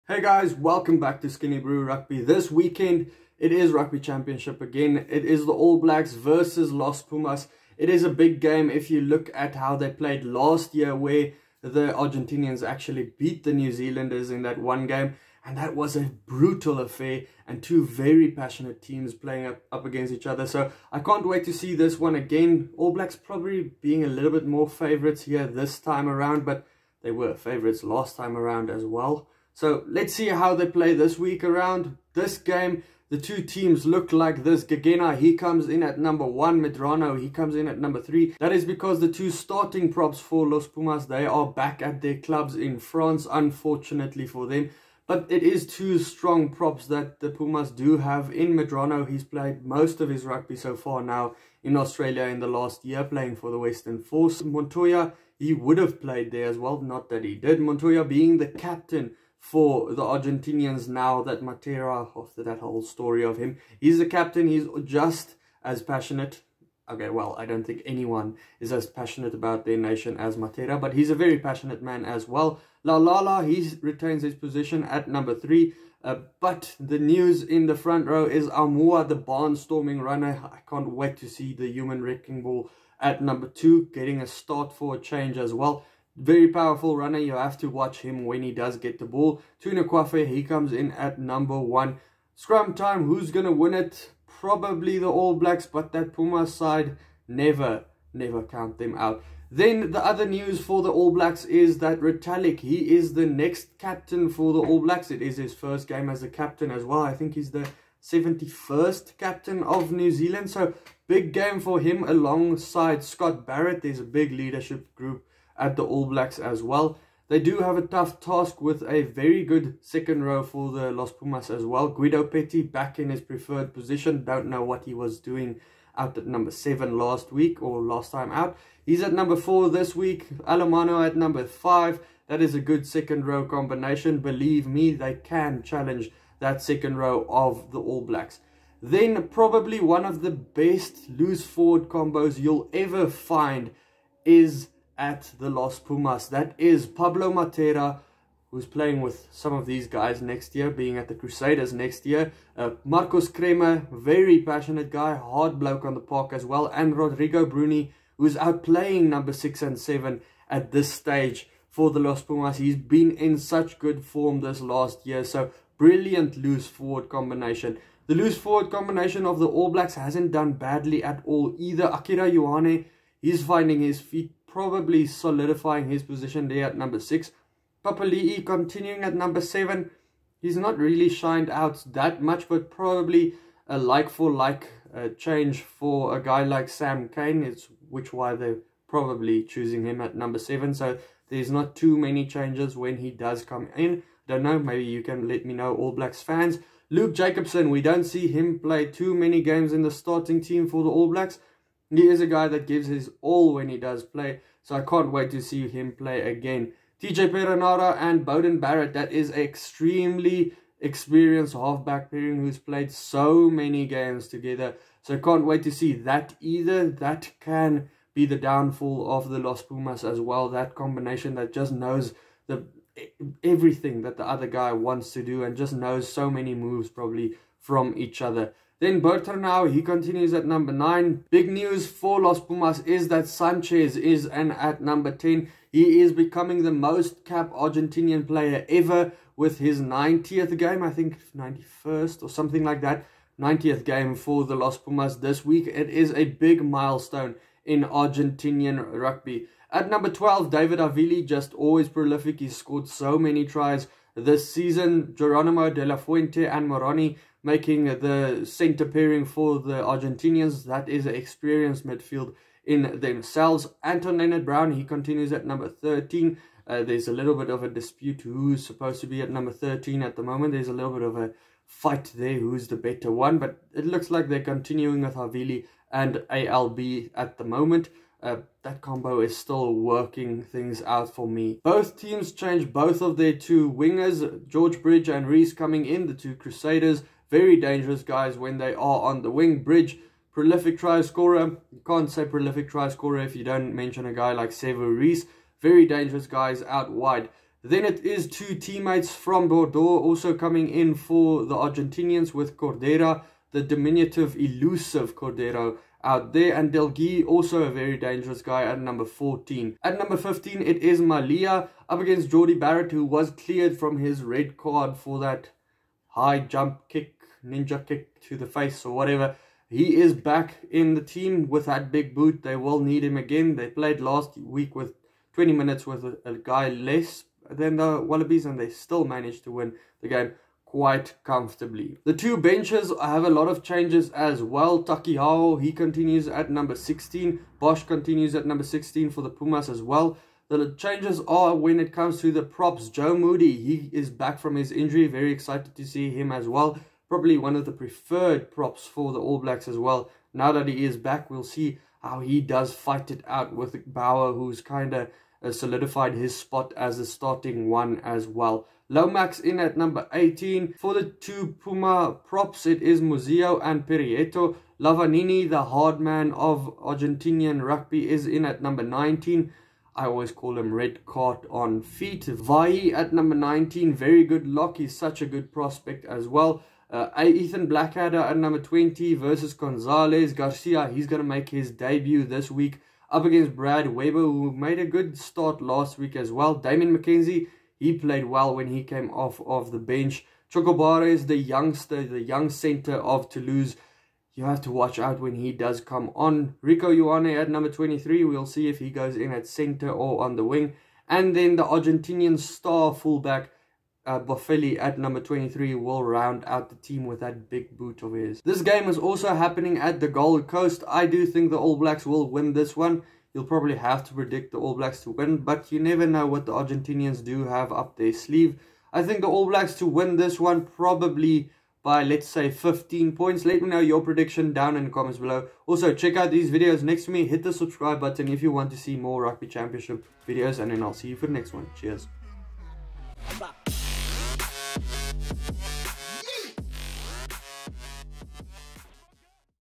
ALL BLACKS vs LOS PUMAS | Round 3 Preview | The Rugby Championship 2021
The Rugby Championship continues this week, with the Los pumas in Australia to play against the All blacks.
Join me as I look closer at the Line-ups and give you my prediction for the game.

(OR use Poolcode: NITSAURA)
(OR use Poolcode: HOODBULL)

00:00 Intro
00:32 Starting 15’s
05:32 Benches
06:48 Prediction